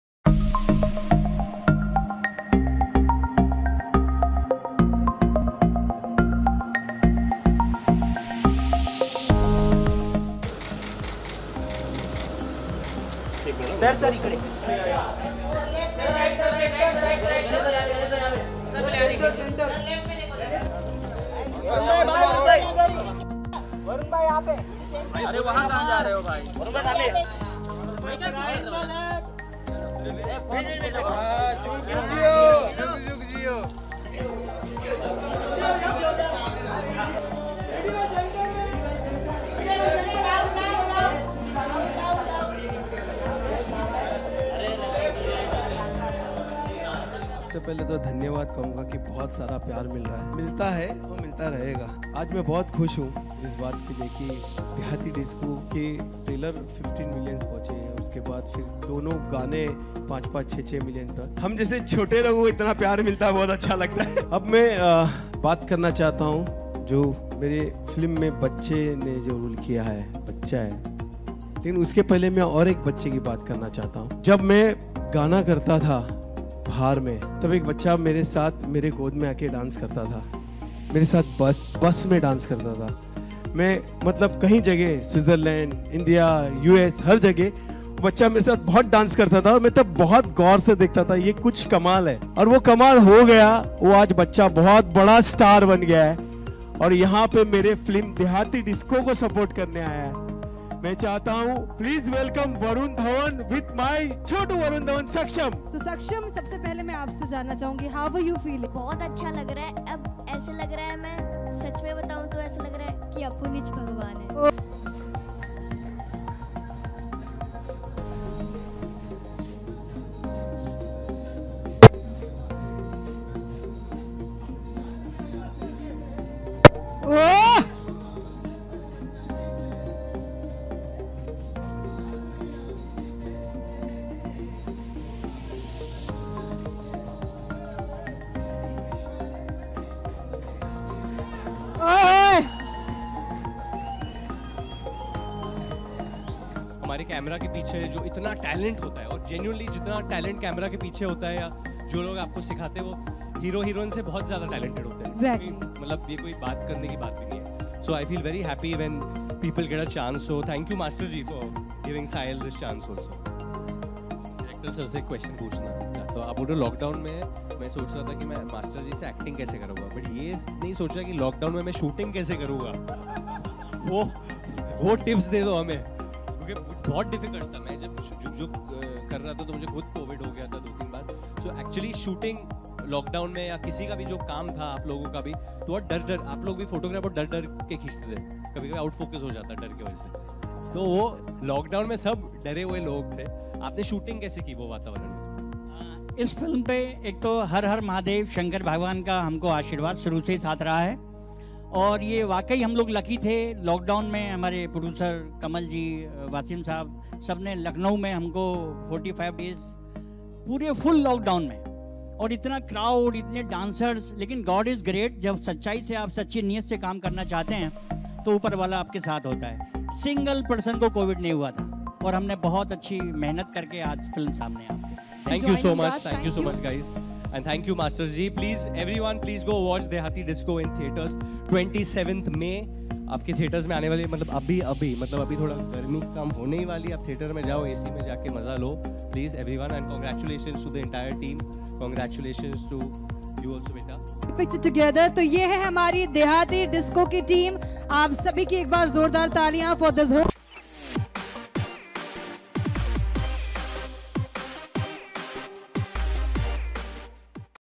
Press Conference Of Film Dehati Disco With Ganesh Acharya, Varun Dhawan & Others | Boogle Bollywood
Copyrights@BoogleBollywood